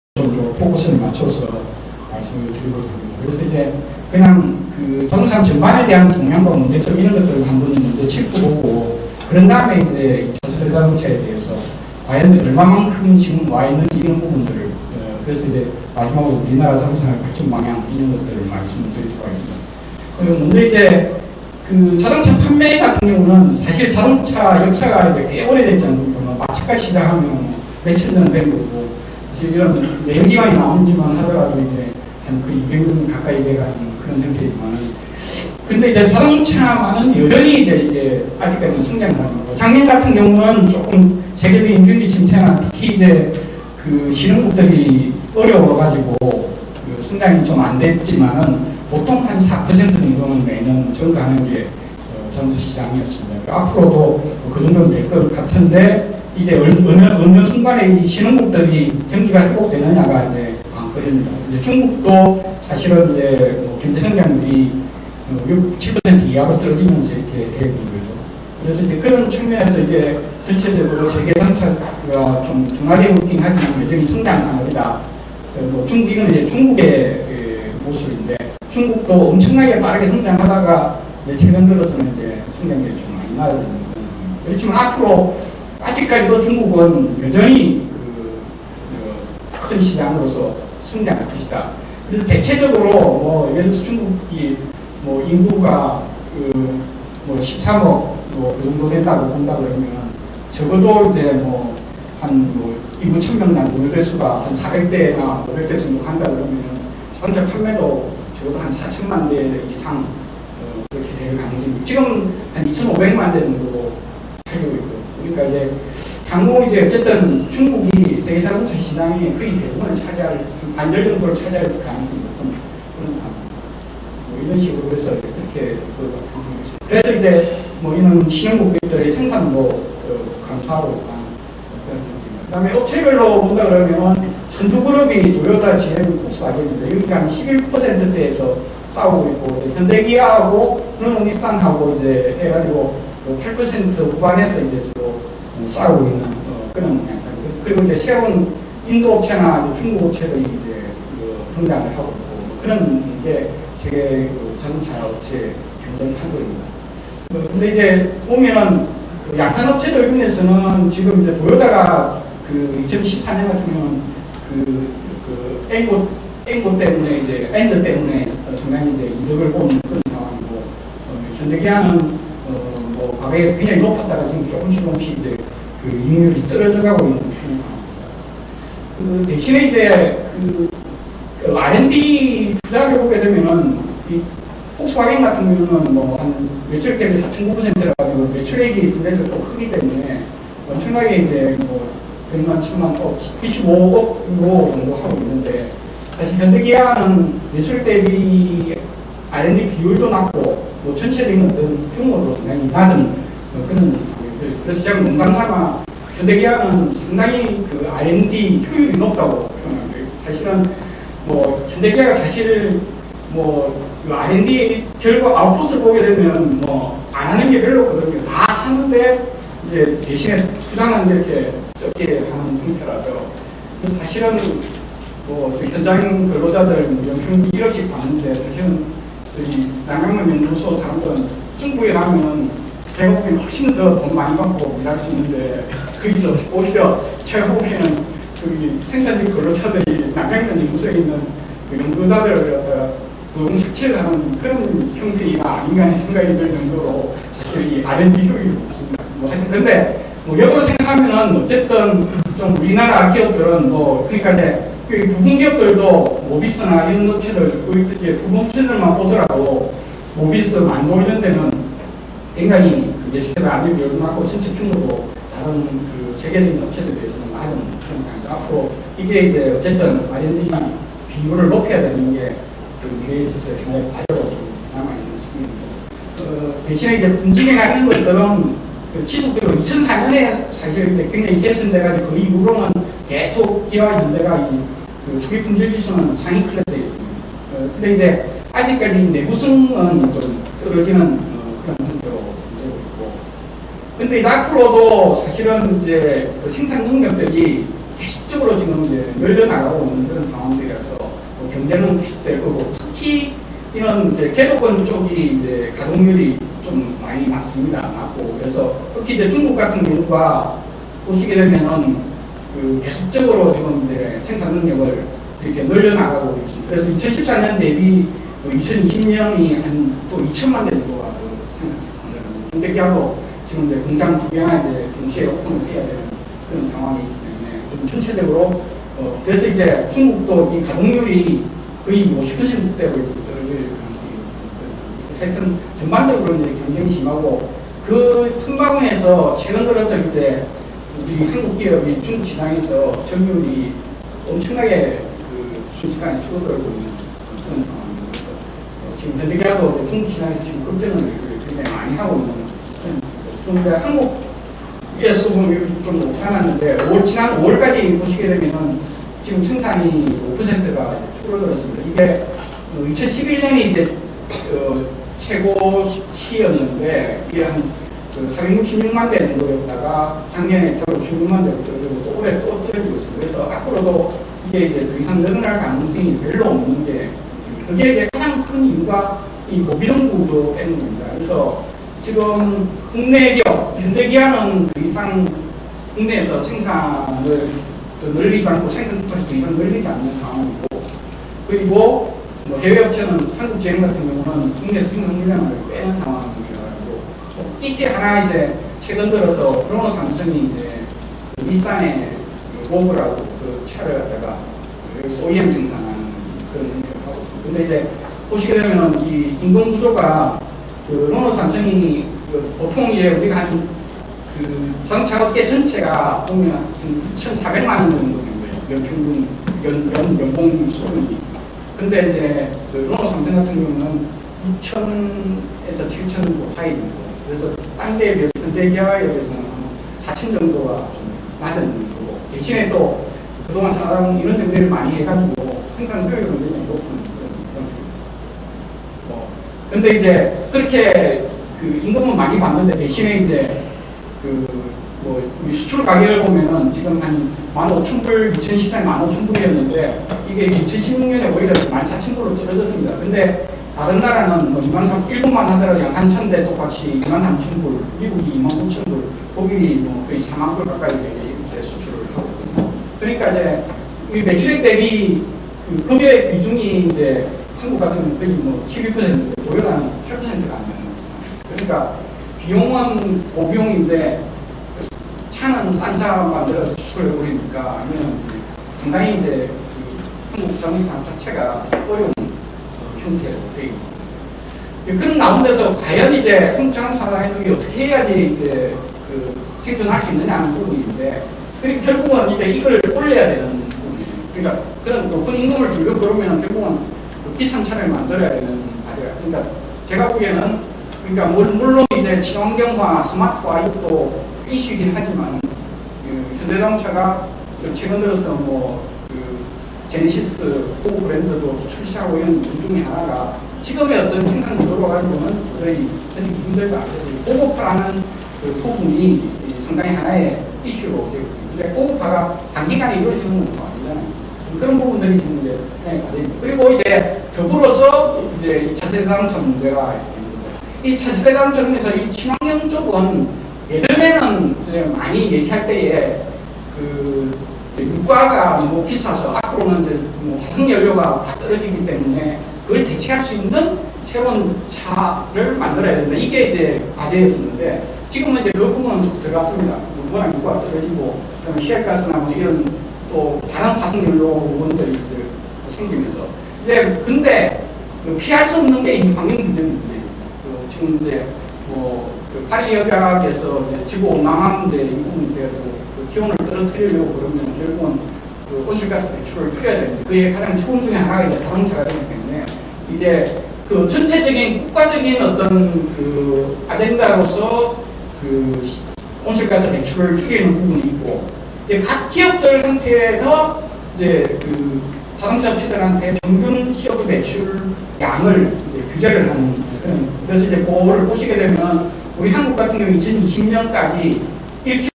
미래자동차산업전략(조철,1/3), KAIST 국가미래전략정기토론회 2016.7.1
주최 : KAIST 문술미래전략대학원
주제 : 미래자동차산업전략
발표 : 조 철 실장 (산업연구원 주력산업연구실)
일시/장소 : 2016년 7월 1일(금) 17:00~19:00 /서울창조경제센터